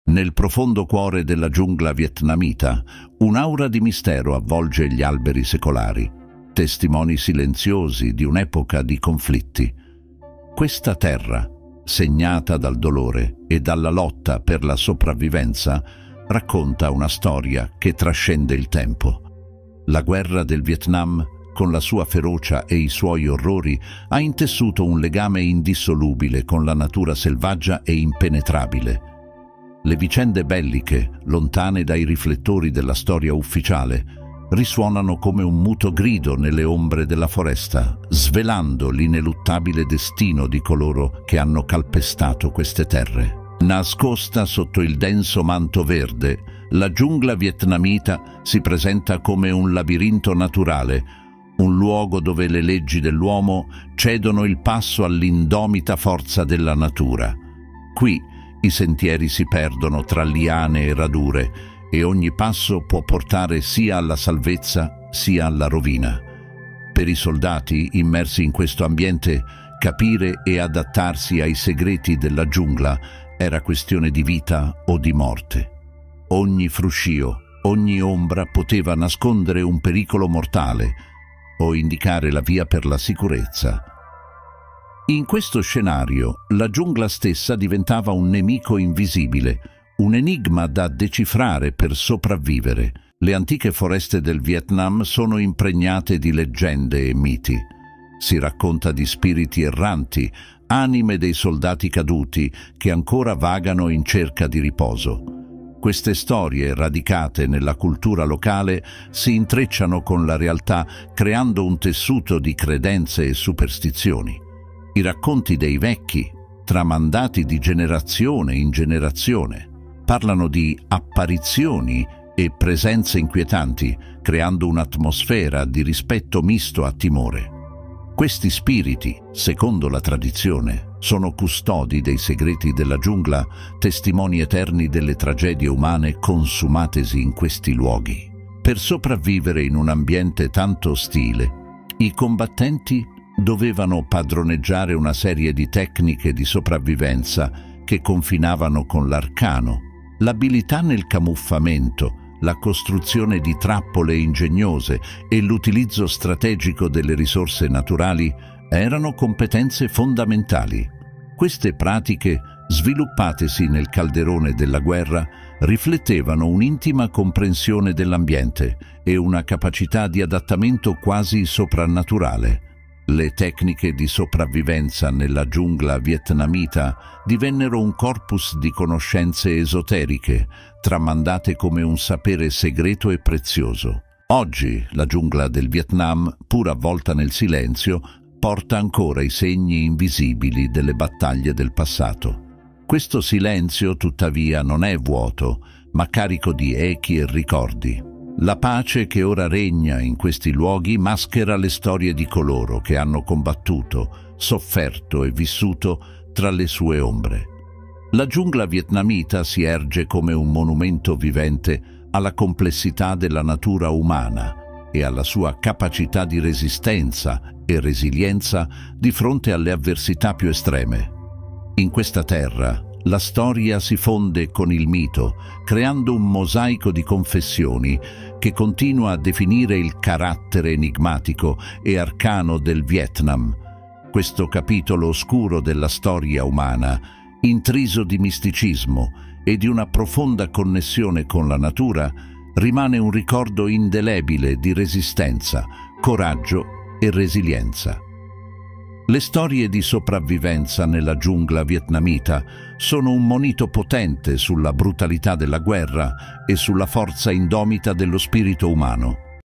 Storie Inedite della Guerra del Vietnam
Descrizione:
Esplora il cuore oscuro e misterioso del Vietnam in una serie affascinante che svela le storie di guerra celate nelle profondità della sua giungla impenetrabile. "Misteri Nascosti: Viaggio nella Giungla Vietnamita" e "Sopravvivenza e Segreti: Storie Inedite della Guerra del Vietnam" ti conducono in un viaggio attraverso sentieri nascosti e paesaggi avvolti nell'ombra del passato. Scopri come soldati e abitanti locali hanno combattuto non solo tra loro, ma anche contro gli elementi implacabili della natura, imparando a decifrare i segreti di una terra tanto bella quanto crudele. Ascolta le leggende di spiriti erranti e tecniche arcane di sopravvivenza che si intrecciano con la storia di una guerra che ha segnato profondamente la terra e le anime di chi l'ha vissuta. Questa narrazione evocativa e ricca di dettagli ti porterà in un viaggio di scoperta, riflessione e profondo rispetto per la complessità della natura umana e della storia.